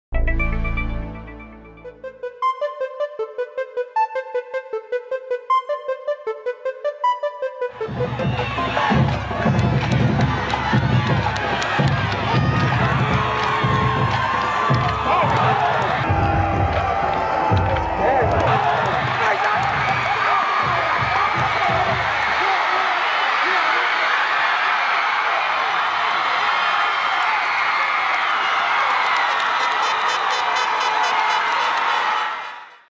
2013 Top 100 Prep Football Plays - No. 13: Destrehan's Rae Juan Marbley strips the defender and scor
Destrehan linebacker and Tulane signee Rae Juan Marbley rips the ball out of the receiver&apos;s hands and scores the touchdown against East St. John